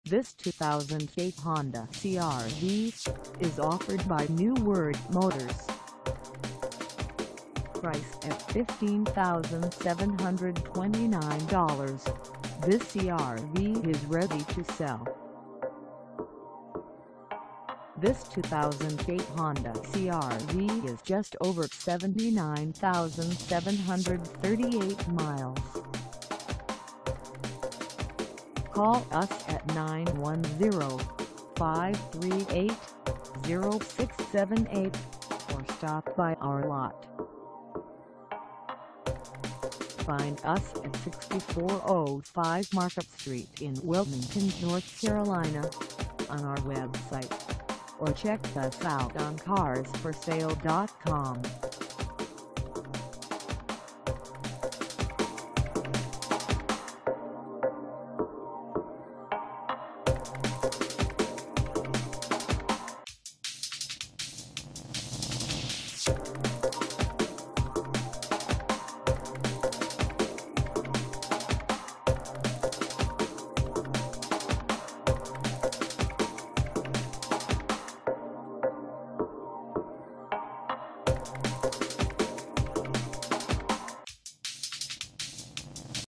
2008 Honda CR-V EX 2WD AT - for sale in Wilmington, NC 28406
Neuwirth Motors
6405 Market St.

Come test drive this 2008 Honda CR-V EX 2WD AT for sale in Wilmington, NC.

All power equipment is functioning properly.            This vehicle has no known defects.          There are no noticeable dings on the exterior of this vehicle.  The engine is mechanically sound and can be driven anywhere with confidence.  Well maintained exterior is extra clean.              This vehicle was well kept, even though it was not garaged.        The inside of this vehicle is smoke-free.        The interior is in excellent condition.      The mileage is accurate.  This vehicle has a smooth shifting transmission.    There are no signs of prior damage on this vehicle.      The tires on this vehicle appear to have no wear at all.